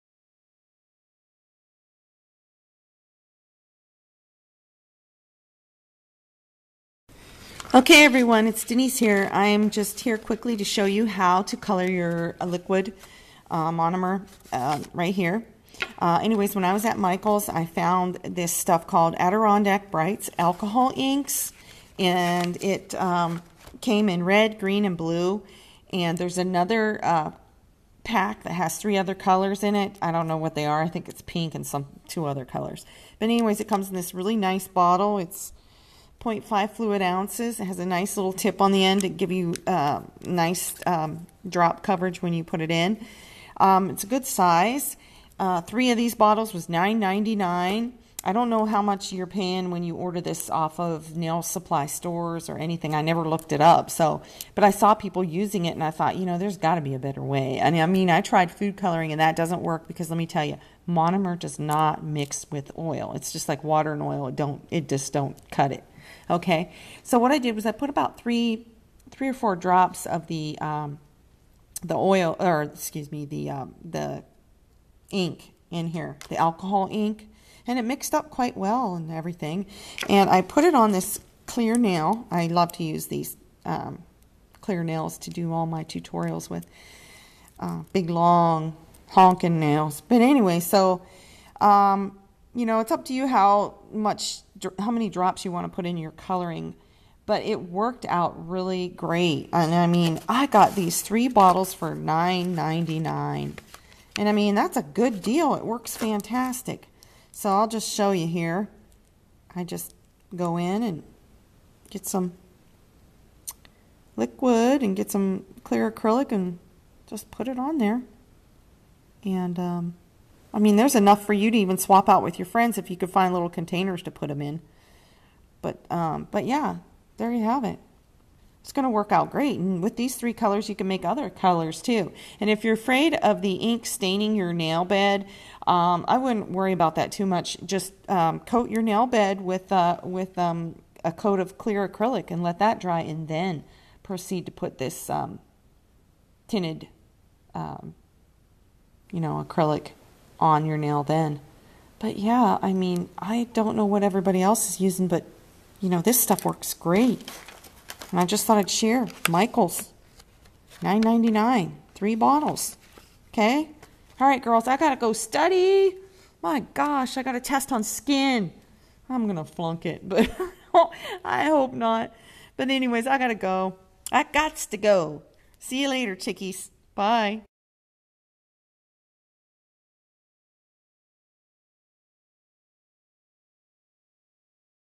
Color your Acrylic Liquid ! EASY/CHEAP! My Nails are UGLY here Too 🤣 !
How to color liquid acrylic . Color your monomer easy .
My Nails are Ugly Here too ! This is a old video . Like maybe 11 years ago . One of my first videos .  I was a beginner in nails . I have really improved over the years . At least I think so .